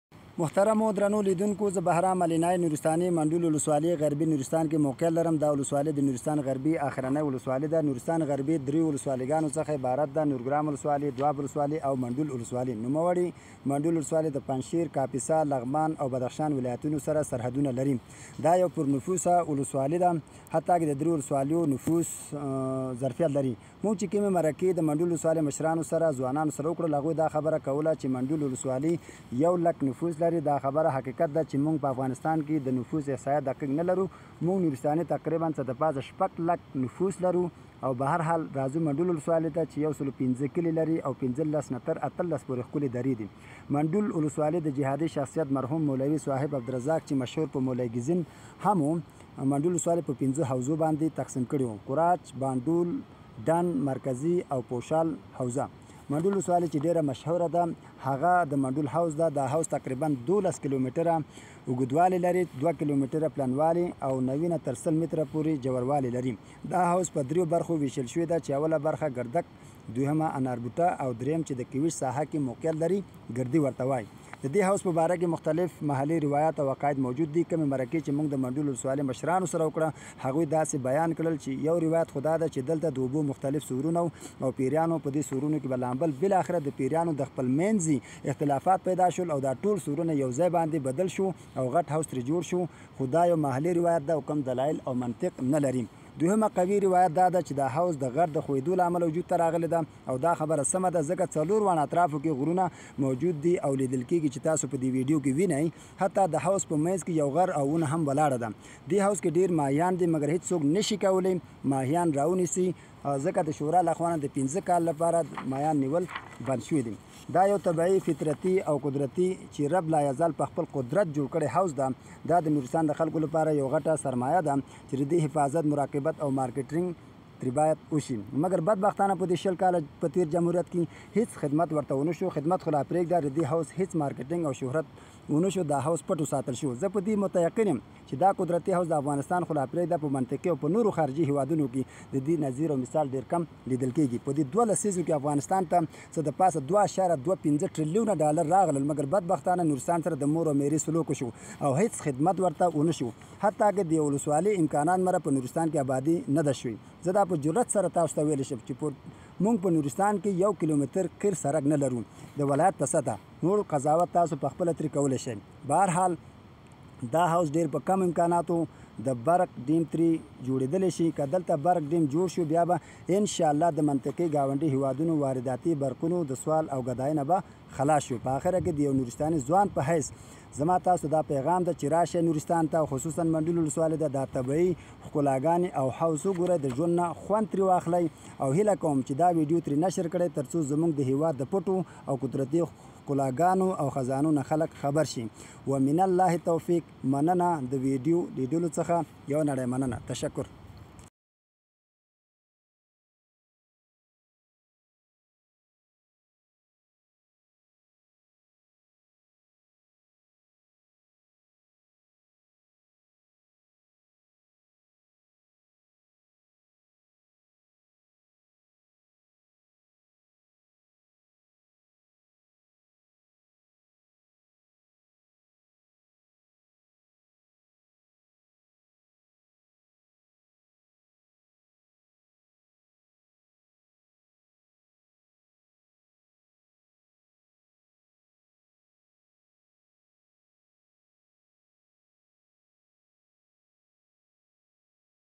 Mandol lake of Nuristan province Afghanistan.
This is the Mandol district, which is located in Western Nuristan. It is the last district of Western Nuristan. As Western Nuristan consists three districts, Nurgram District, Doab District and lastly Mandol District.
This Mandol district has borders/ boundaries with Panjshir, Kapisa, Laghman and Badakhshan provinces. This district has a huge population, of one lakh (100,000). It is true and fact that in Afghanistan, yet we do not have accurate census/statistics in Afghanistan. We Nuristanis are more than 6 lakh in Afghanistan, but Mandol district has 105 villages and 15 to 18 beautiful valleys.
Mandol district was divided into 5 parts/sections Korach part, Bandol part, Dand part, Central part and Poshal part.
The most famous of Mandol district is the Mandol the longest, the tallest and the most beautiful lake. Approximately/ more or less 12 kilometers are long,2 kilometers wide and 90 to 100 meters depth.
This famous lake is divided into three parts, the first part is called Gardag, that this the upper side the second is Anarbuta which is middle part and the third is Gardag, which is located the bottom of the Kevisht area.
There are different beliefs about this lake.The fact about this lake is this, due to the erosion/ dissolution of the mountains which covered by four sides came into existence, and this is true because there are mountains in all four sides and you can watch a tree standing on the mountain which is in the middle of the lake. There are many fish in this lake. But no one can fishing because the council has banned/ prevented.
This is a natural, beautiful and God built lake. This is a great an asset for the people of Nuristan. Therefore, it is our responsibility to protect, preserve, and to do the marketing of this lake for future an investment.
However, look at this lake, which can be built a large hydro-power electricity dam, with less possibilities/investment. If a hydro-power dam is built here, then hopefully we shall be self-sufficient and freed from the demand of imported electricity, from the neighboring countries, especially from the Central Asia.
At the end, as a young Nooristani/ Alinain guy, I have this message for you ladies and gentlemen, visit to Nooristan, especially to Mandol district, to see the beautiful places and natural resources of Nuristan and enjoy your life.